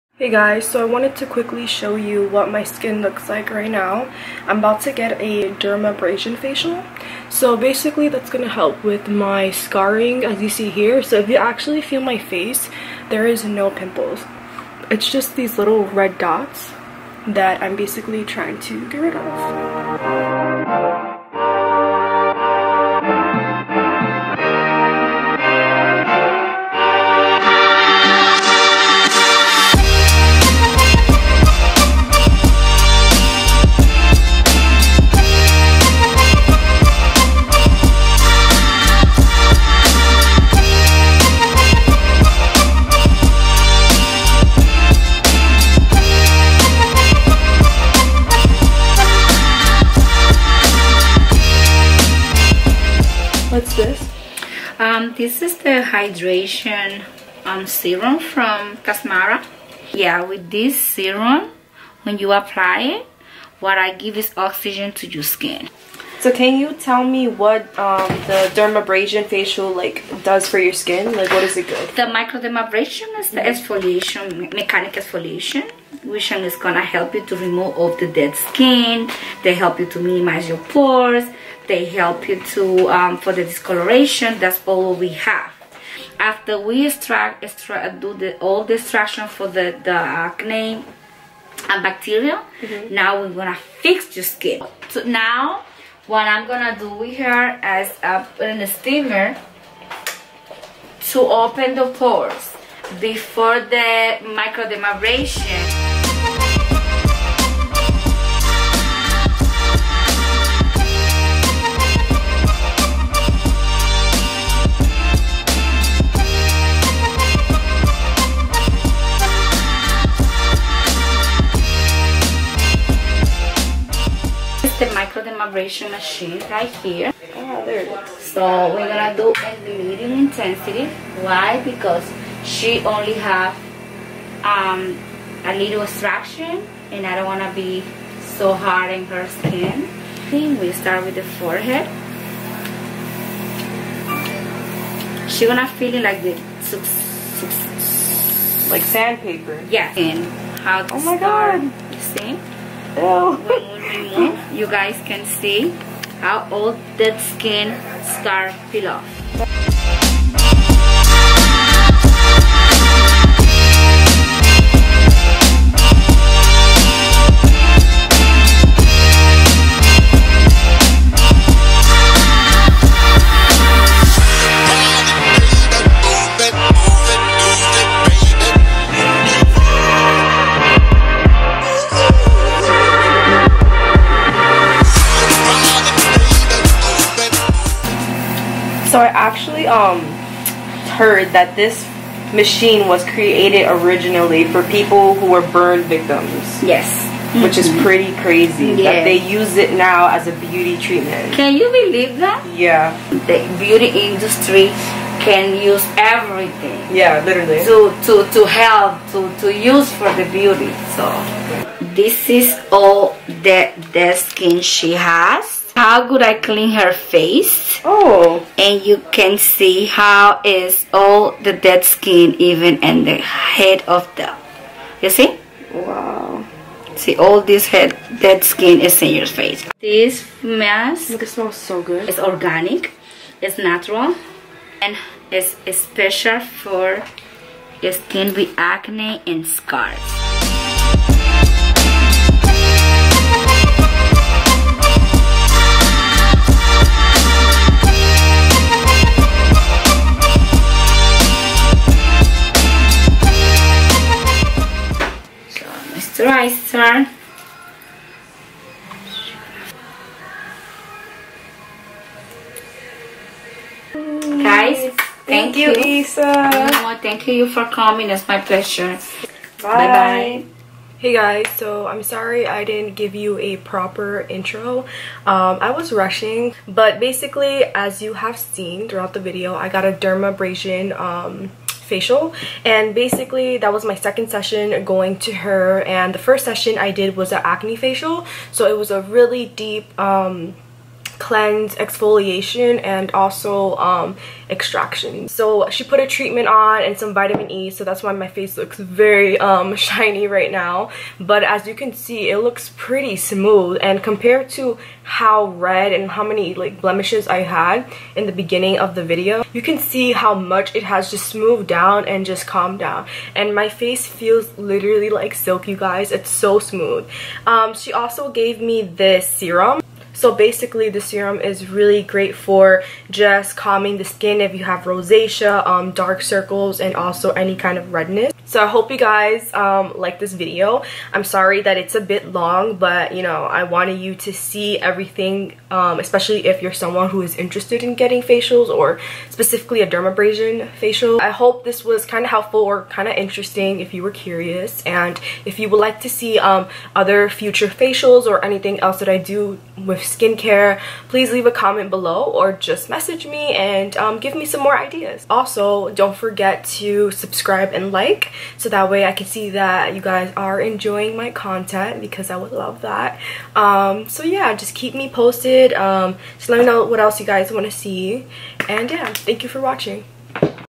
Microdermabrasion Facial | Acne Prone Skin
Hey babes! In this video I take you with me to get my first Microdermabrasion facial! You will learn about the benefits of this facial and how it works in person.

During this pandemic my face had broken out very bad because of the material of the surgical mask, but once I invested in getting facials at least 1-2 times a month my skin has drastically changed and I rarely ever get pimples now! I also have to wear a 100% cotton mask under the surgical mask to prevent my skin from getting irritated again. I hope this video, and my tips help you out if you also have acne skin & are curious about this facial!

My cosmetic business insta: @helena.beautyy